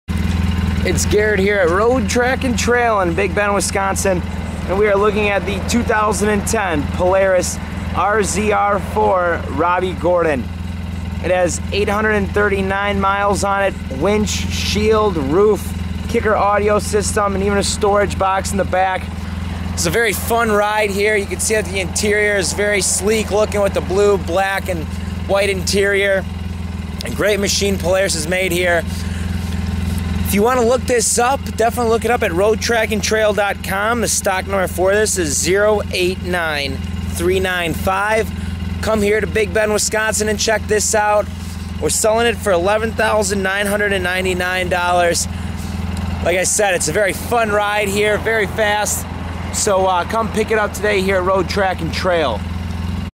2010 Polaris RZR4 Robby Gordon $11,999 at Road Track & Trail
SHIELD, WINCH, ROOF, KICKER AUDIO SYSTEM, LED'S, STORAGE BOX, A-ARM GUARDS, EXTRA GAUGES!!!
Stock #089395 at Road Track & Trail, Big Bend WI